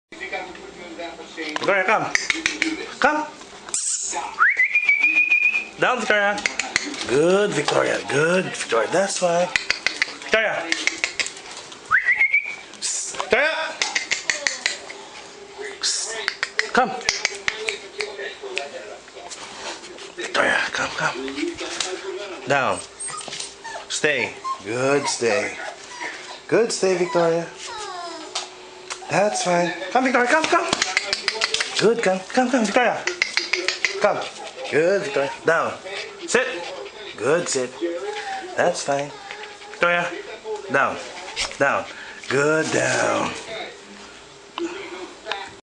siberian husky on stairs.flv
my 3 months siberian husky victoria practicing walking on stairs,